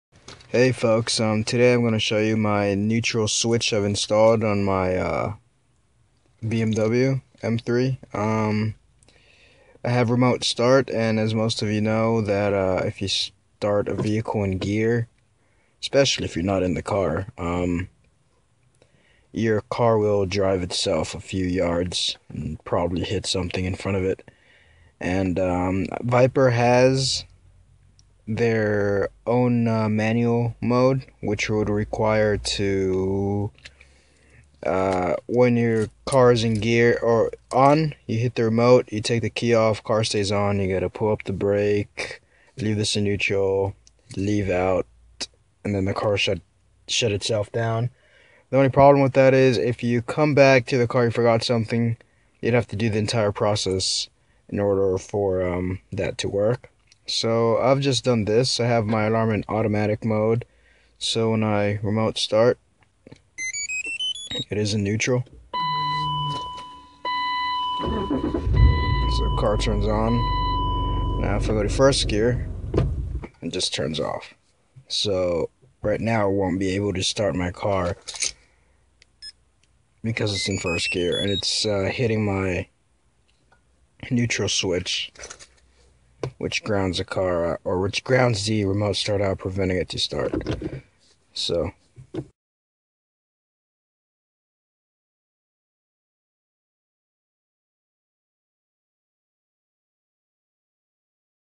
Remote Start on Manual Stick Shift Car w/ Neutral Safety Switch
Basically what i did was i got a few Momentary Push Switch (Normally Closed). Momentary is a type of spring loaded switch that will temporary provide power (Normally Open) or temporary disrupt power (Normally Closed) to a circuit. I wired them to the to the "Neutral Safety Switch Input (-)" on the Remote Start brain. This wire is normally hooked on with an On/Off Switch that is provided by Viper so that you can manually turn the Remote Start feature on or off. The wire is Grounded, so when the Switch is OFF, it will disrupt the ground, the Remote Start brain will see that there is no ground so it will not start the car. I just tapped the Momentary Switches into this wire After the On/Off switch. Made a custom Bracket (look at End of video), and BAM,

I used Radio Shacks  Model # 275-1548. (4 in a pack).

You can use a Normally Closed reed with a magnet on the shifter as well.

NOTE: In order to use the Reed Switches that are used in home alarms you will need to wire it us to your Remote Start Hood switch (or something that is Normally Open).